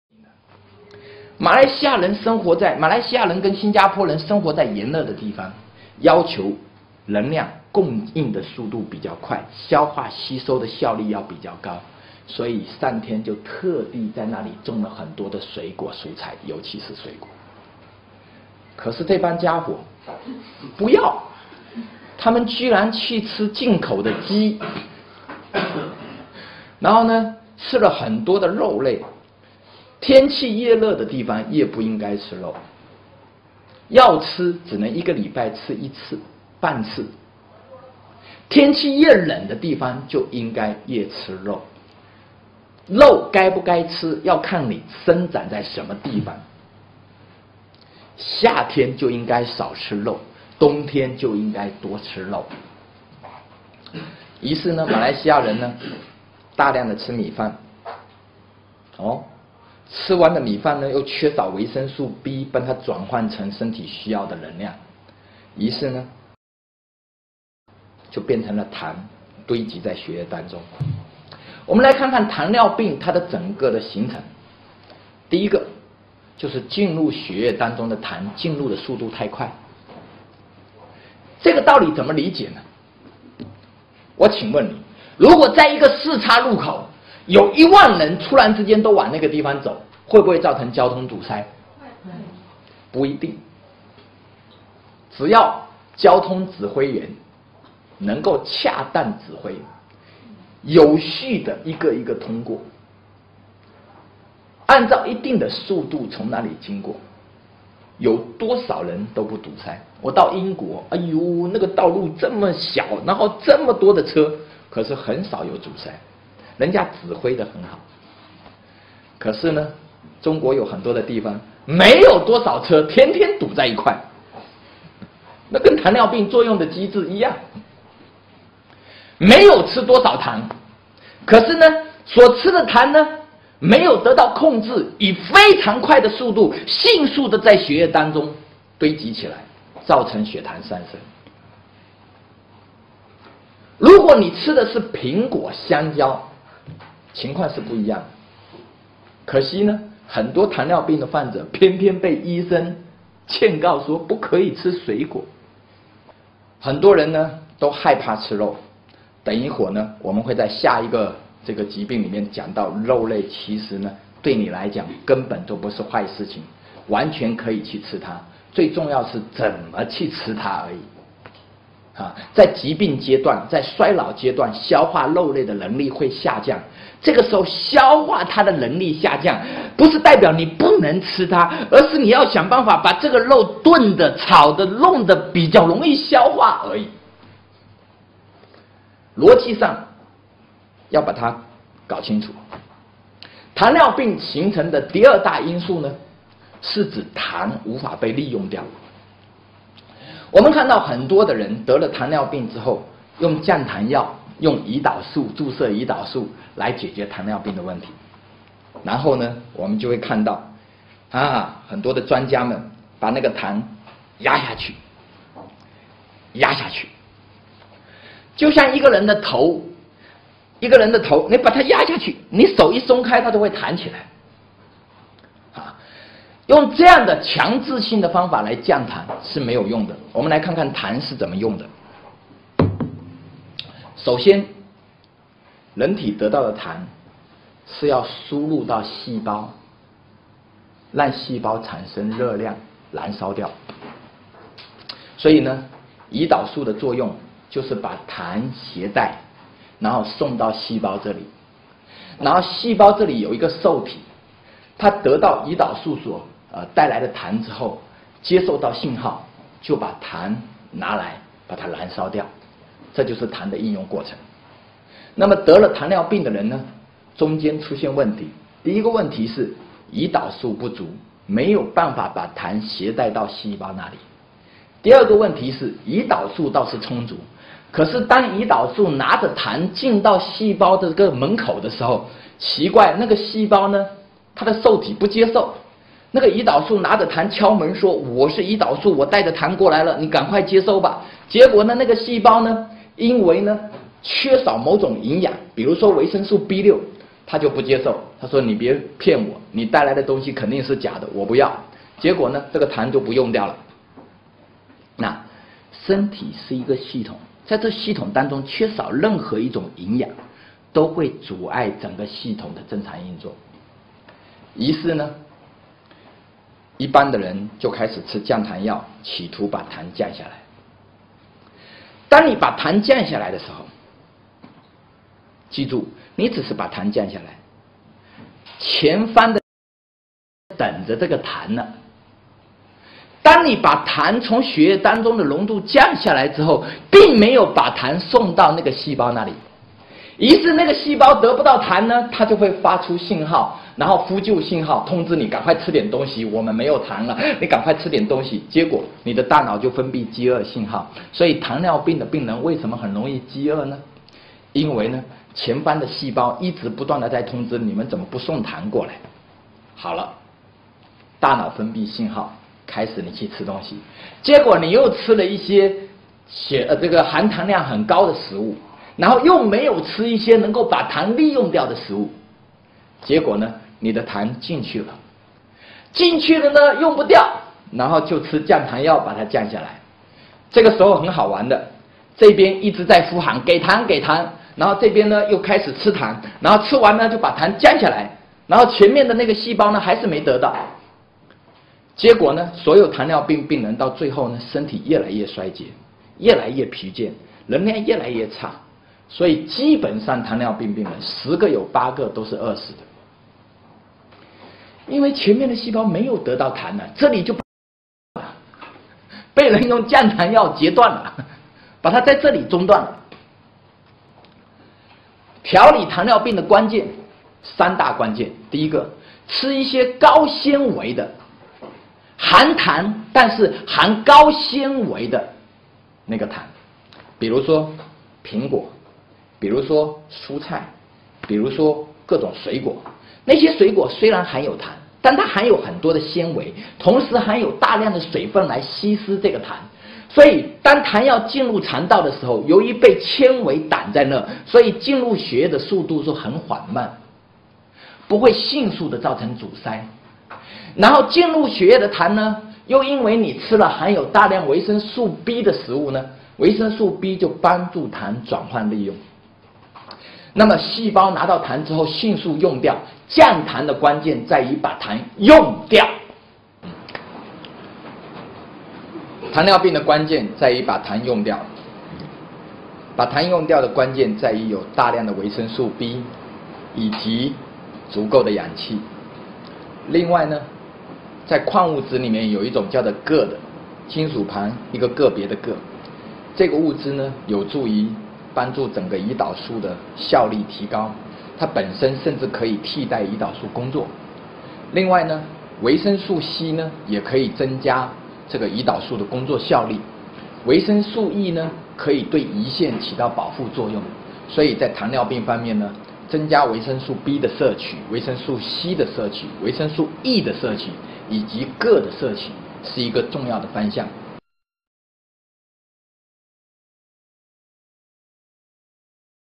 糖尿病形成的原因及如何修復? - 林海峰營養師
糖尿病為何是不治之症, 原因就是.
糖尿病形成的原因及如何修復…